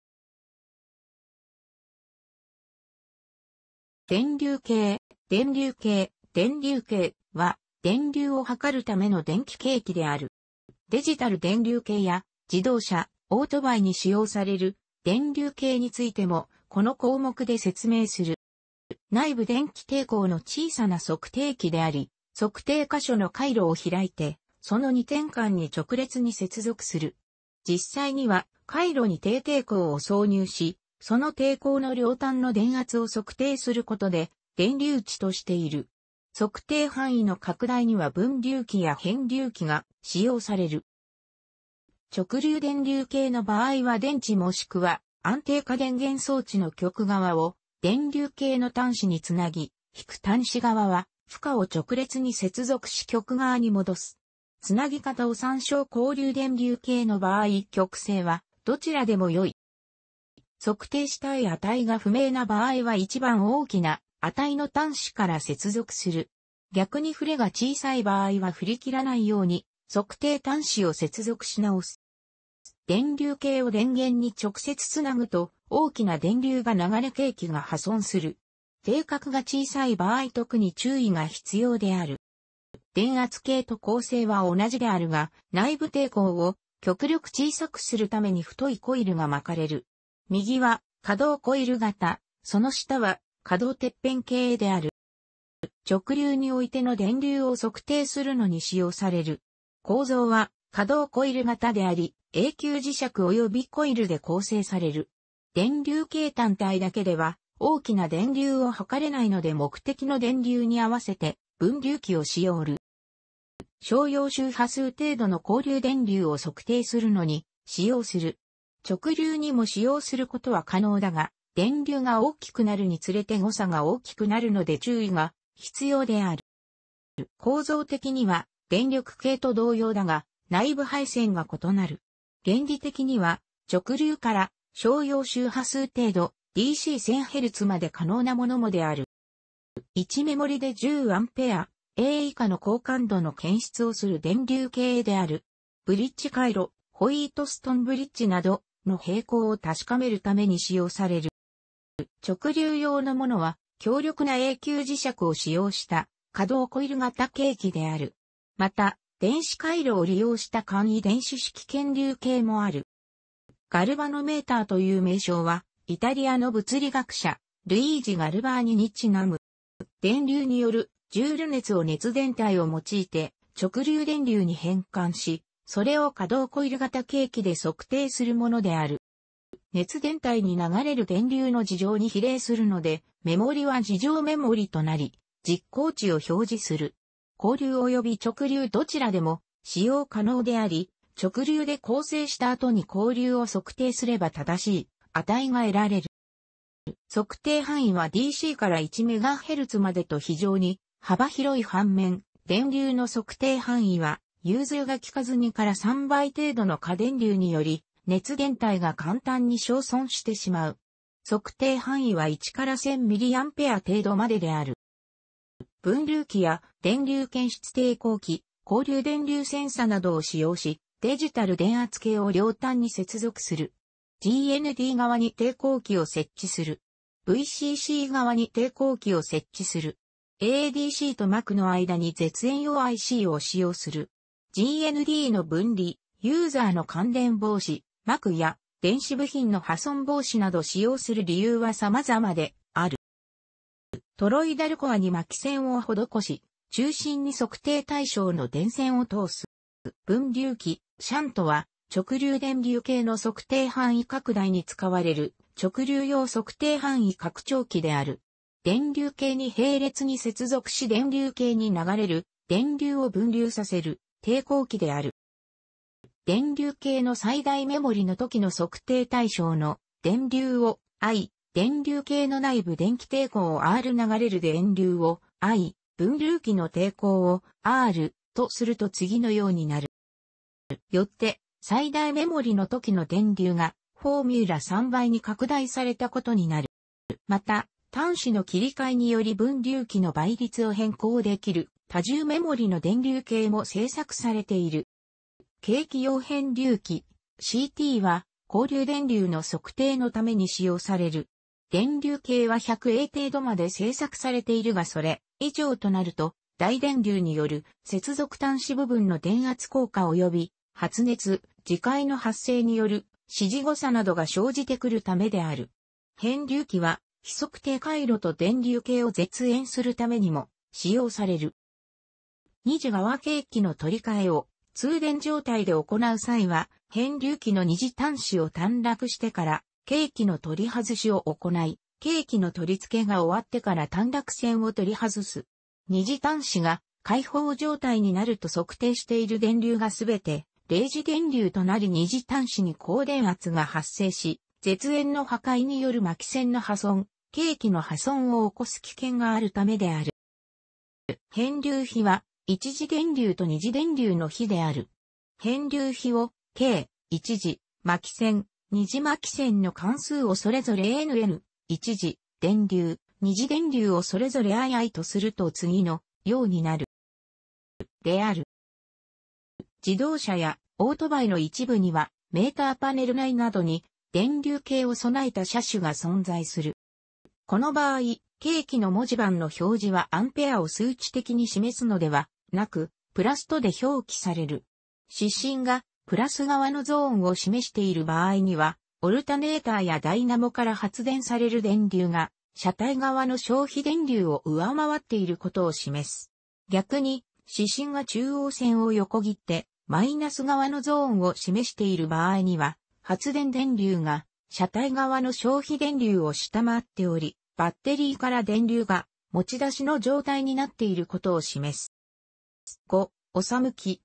電流計
電気・電子計測機器

電流計（でんりゅうけい、）は、電流を測るための電気計器である。

デジタル電流計や自動車、オートバイに使用される電流計についても、この項目で説明する。

内部電気抵抗の小さな測定器であり、測定箇所の回路を開いてその2点間に直列に接続する。実際には、回路に低抵抗を挿入し、その抵抗の両端の電圧を測定することで電流値としている。測定範囲の拡大には分流器や変流器が使用される。

直流電流計の場合は電池もしくは安定化電源装置の＋極側を電流計の＋端子につなぎ、－端子側は負荷を直列に接続し－極側にもどす。（つなぎ方を参照）
交流電流計の場合極性はどちらでも良い。

測定したい値が不明な場合は一番大きな値の端子から接続する。逆に振れが小さい場合は振り切らないように測定端子を接続し直す。

電流計を電源に直接つなぐと大きな電流が流れ計器が破損する。定格が小さい場合特に注意が必要である。

電圧計と構成は同じであるが、内部抵抗を極力小さくするために太いコイルが巻かれる。右は可動コイル形、その下は可動鉄片形である。

直流においての電流を測定するのに使用される。構造は可動コイル型であり永久磁石およびコイルで構成される。電流計単体だけでは大きな電流を測れないので目的の電流にあわせて分流器を使用する。

商用周波数程度 (45 - 65Hz) の交流電流を測定するのに使用する。直流にも使用することは可能だが、電流が大きくなるにつれて誤差が大きくなるので注意が必要である。

構造的には電力計と同様だが内部配線が異なる。原理的には直流から商用周波数程度（DC - 1000Hzまで可能なものも）である。

一目盛りで10アンペア[A]以下の高感度の検出をする電流計である。ブリッジ回路（ホイートストンブリッジなど）の平衡を確かめるために使用される。

直流用のものは、強力な永久磁石を使用した可動コイル形計器である。また、電子回路を利用した簡易電子式検流計もある。

ガルバノメーターという名称は、イタリアの物理学者、ルイージ・ガルヴァーニにちなむ。

電流によるジュール熱を熱電対を用いて直流電流に変換し、それを可動コイル形計器で測定するものである。

熱電対に流れる電流の二乗に比例するので目盛りは二乗目盛りとなり実効値を表示する。交流および直流どちらでも使用可能であり、直流で校正した後に交流を測定すれば正しい値が得られる。

測定範囲はDC - 1MHzまでと非常に幅広い反面、電流の測定範囲は融通が利かず2 - 3倍程度の過電流により熱電対が簡単に焼損してしまう。測定範囲は1 - 1000mA程度までである。

分流器や電流検出抵抗器、交流電流センサなどを使用しデジタル電圧計を両端に接続する。
　GND側に抵抗器を設置する。
　Vcc側に抵抗器を設置する。
　ADCとMCUの間に絶縁用ICを使用する。GNDの分離、ユーザーの感電防止、MCUや電子部品の破損防止など使用する理由は様々である。
トロイダルコアに巻線を施し、中心に測定対象の電線を通す。

分流器（シャント）は、直流電流計の測定範囲拡大に使われる直流用測定範囲拡張器である。電流計に並列に接続し電流計に流れる電流を分流させる抵抗器である。

電流計の最大目盛りの時の測定対象の電流をI、電流計の内部電気抵抗をr・流れる電流をI、分流器の抵抗をR、とすると次のようになる。
よって、最大目盛りの時の電流がformula_3倍に拡大されたことになる。

また、端子の切り替えにより分流器の倍率を変更できる多重目盛りの電流計も製作されている。

計器用変流器（けいきようへんりゅうき）、CTは交流電流の測定のために使用される。電流計は100A程度まで製作されているがそれ以上となると、大電流による接続端子部分の電圧降下及び発熱、磁界の発生による指示誤差などが生じてくるためである。（変流器は被測定回路と電流計を絶縁するためにも使用される）

二次側計器の取替えを通電状態で行う際は、変流器の二次端子を短絡してから計器の取り外しを行い、計器の取り付けが終わってから短絡線を取り外す。二次端子が開放状態になると測定している電流が全て励磁電流となり二次端子に高電圧が発生し、絶縁の破壊による巻線の破損、計器の破損を起こす危険があるためである。

変流比は、一次電流と二次電流の比である。変流比を "K" 、一次巻線、二次巻線の巻数をそれぞれ "N" ・ "N" 、一次電流、二次電流をそれぞれ "I" ・ "I" とすると次のようになる。

である。

自動車...